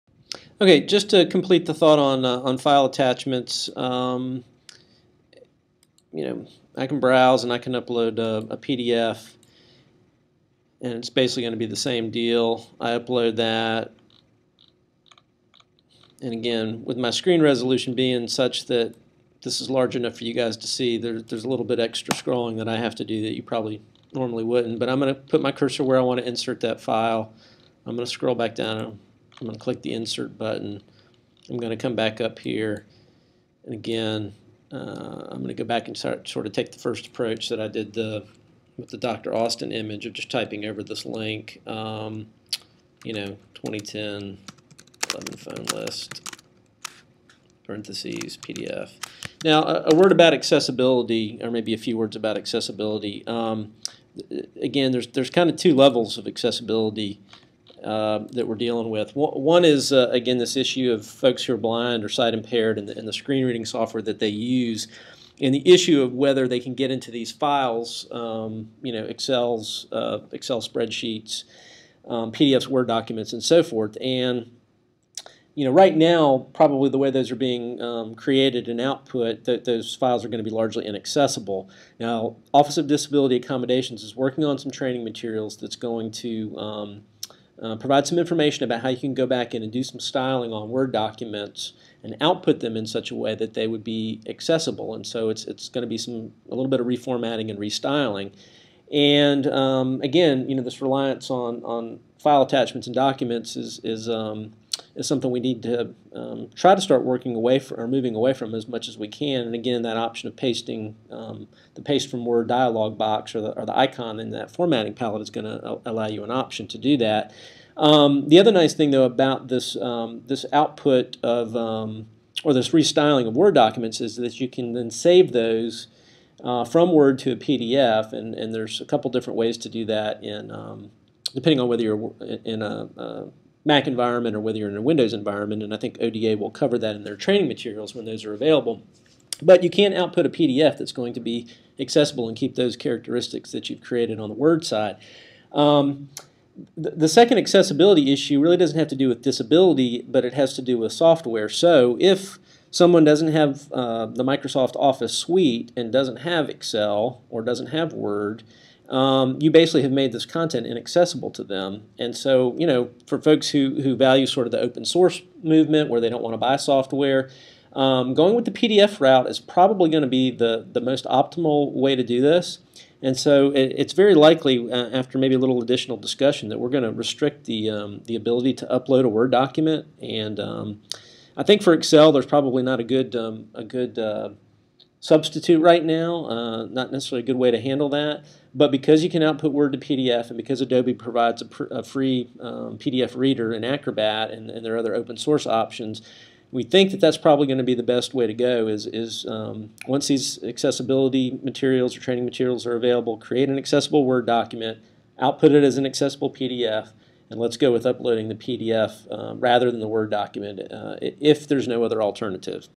Video 14 - Page (File Attachments & Accessibility)
A recap of file attachments and accessibility issues related to using attached files instead of text in pages. Accessibility can be an issue both from a disability standpoint (sight-impaired users) as well as from a software standpoint (users who don't own a copy of Microsoft Office).

The Office of Disability Accommodations is working on training materials for making Word/PDF files accessible. But using PDFs is recommended from a software-accessibility standpoint.